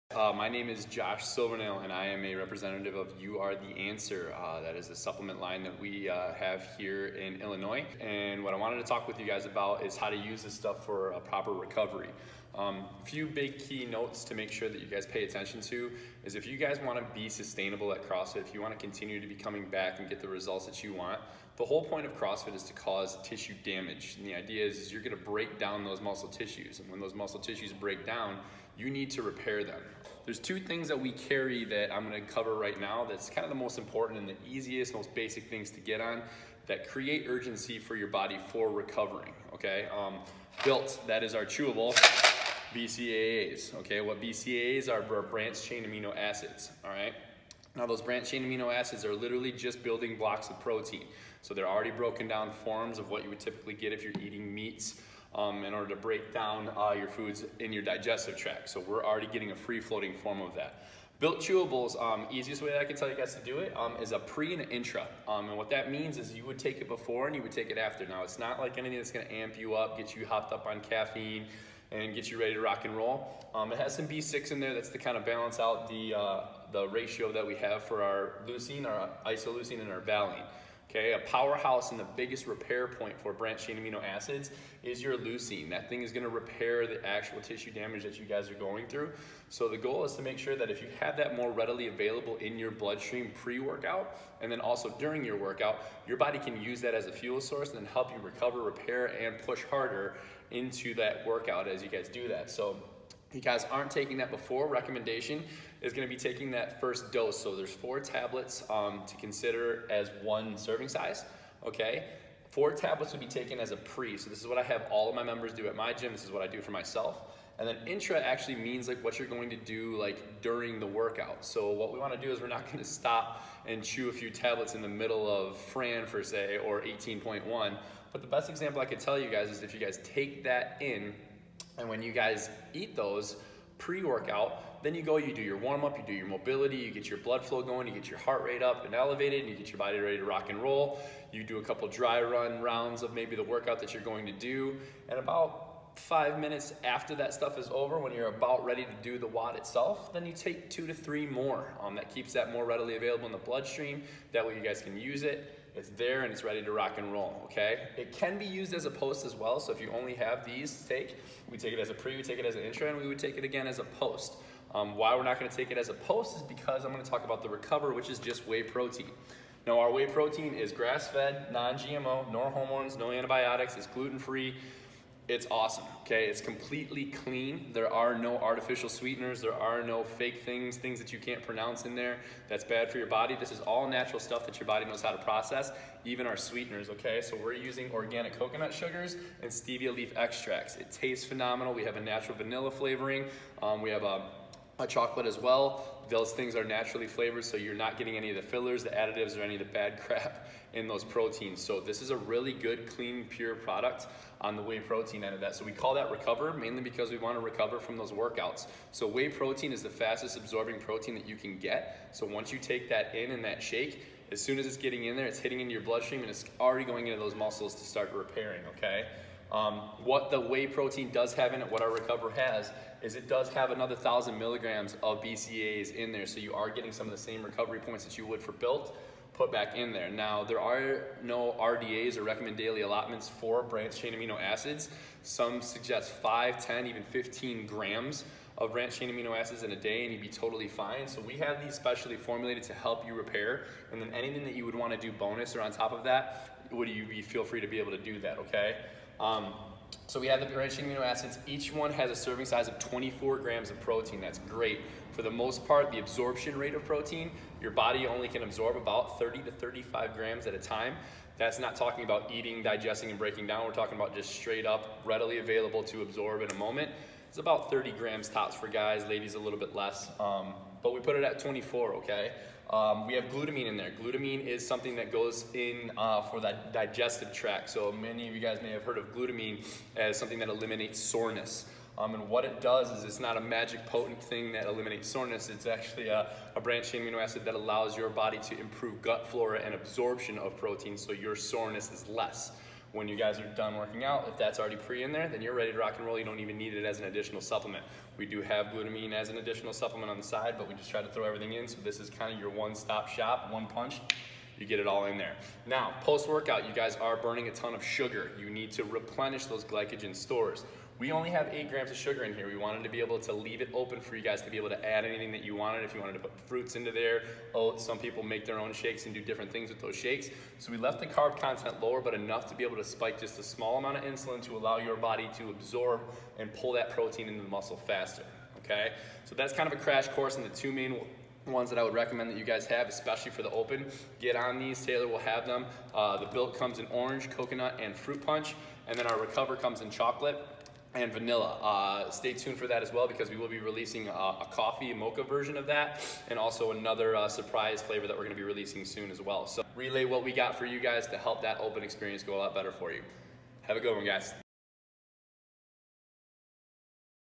Dialing in for recovery
Learn how to use protein and BCAA's for more complete and faster recovery.